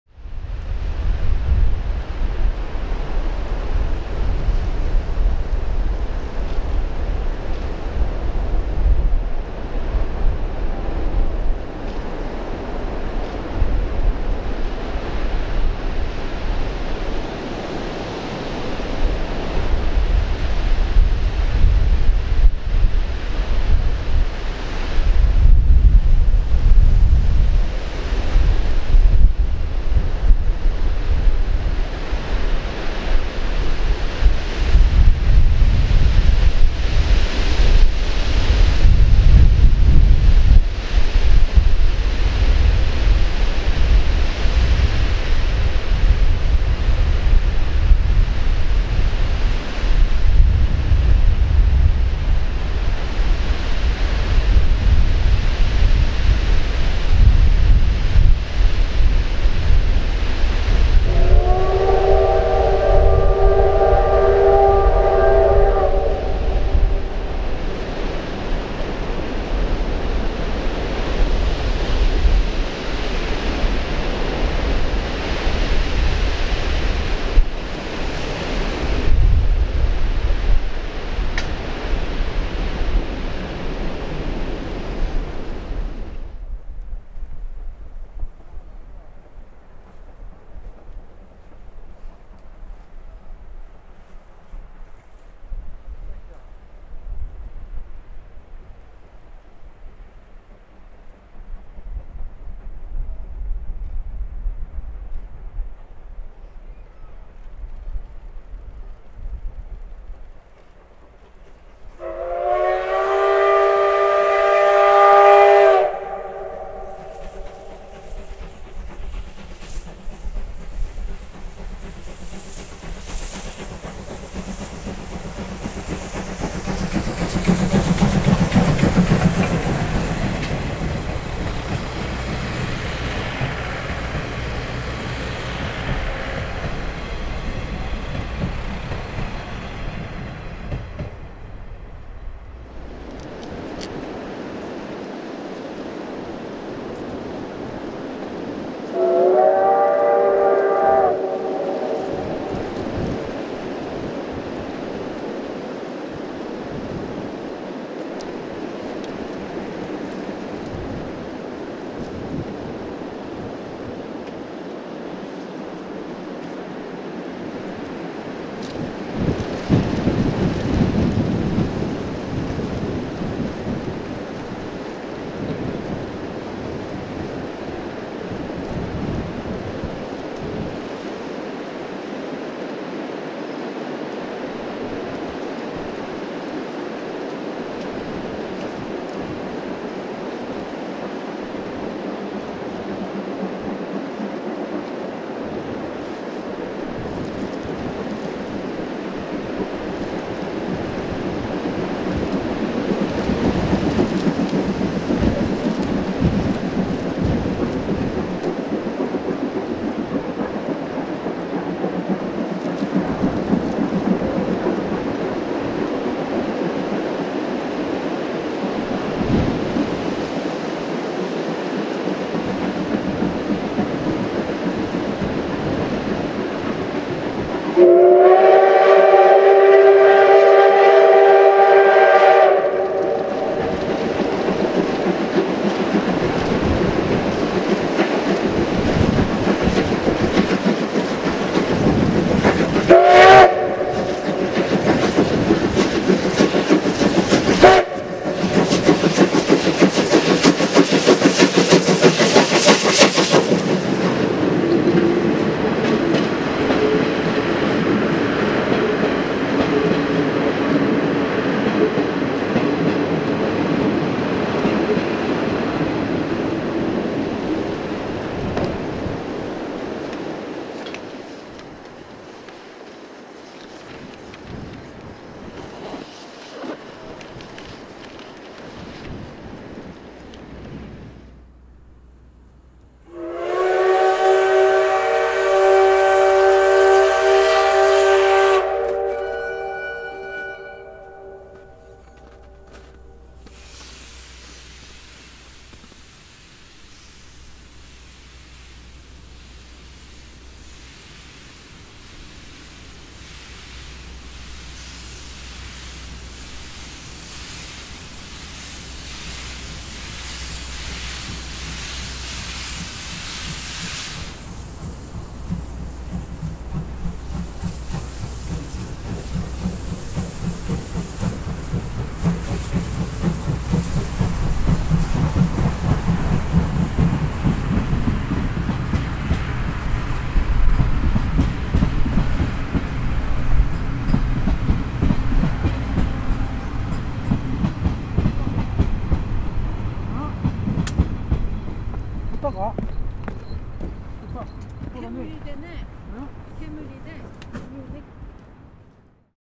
みちのくSLギャラクシー号 釜石線送り込み回送 2014年3月6日
ネットで公開される写真のすごさに、どうにも我慢できずに行ってきました。花巻→-釜石の送り込み回送です。
爆弾低気圧のせいで、冬の熱水のような暴風が吹き荒れ参りましたが、ほぼ定刻で動いてくれてホッとしました。